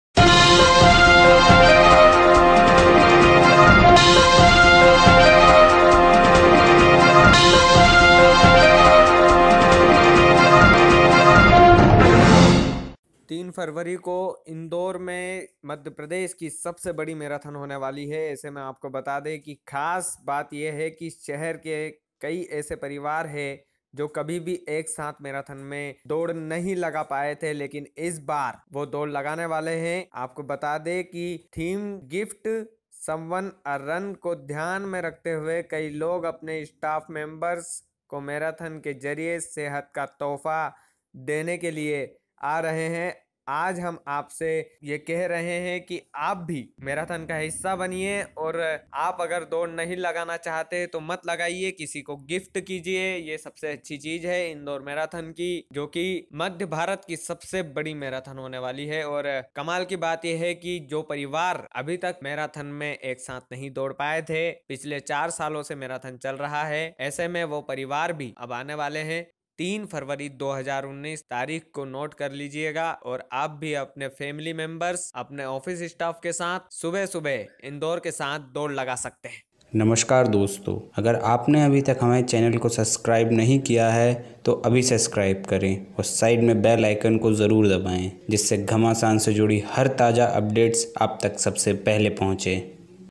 इस बार परिवार के साथ Marathon में दौड़ेगा Indore | This time the Family will run in Indore Marathon
इस Marathon race में लगभग 25,000 लोग दौड़ेंगे | About 25,000 people will run in this Marathon race